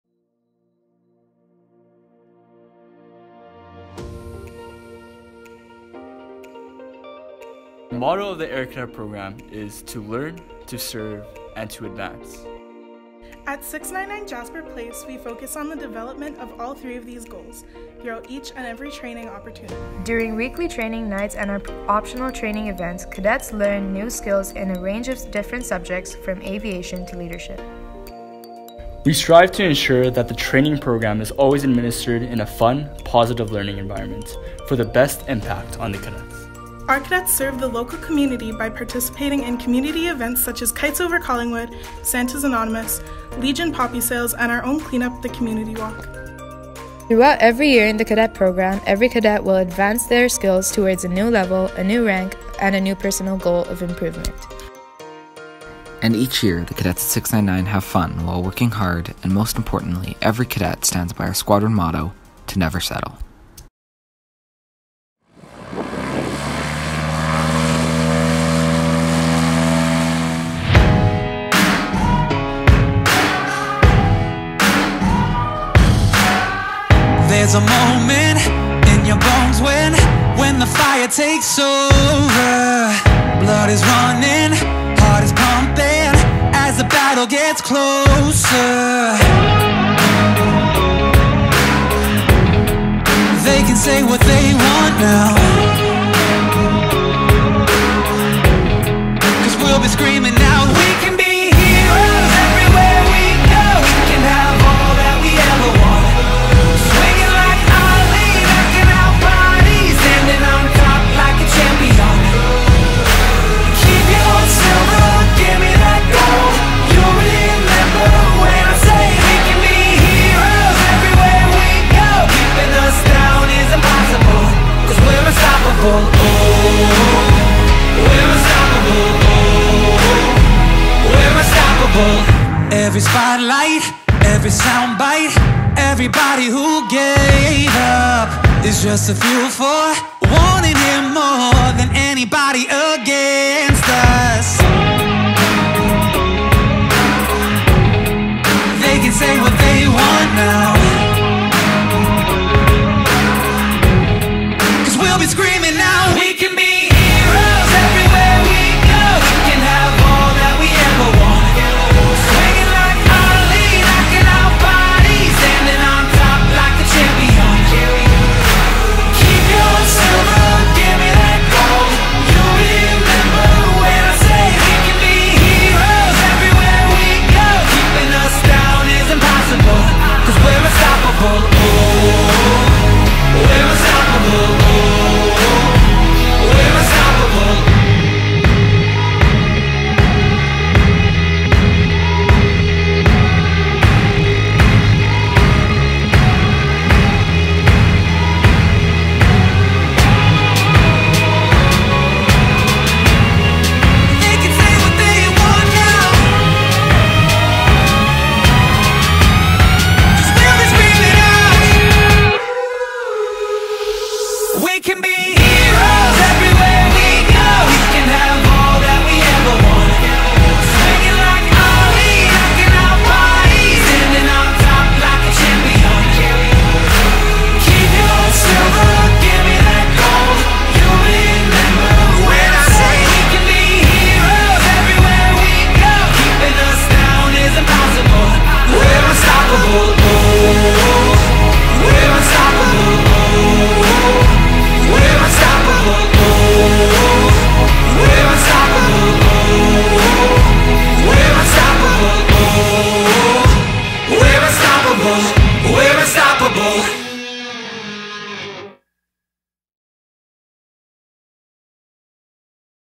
2018-19 699 Year In Review
Each year at 699 the cadets participate in many different activities and this video was put together to help showcase some of the events the cadets have participated in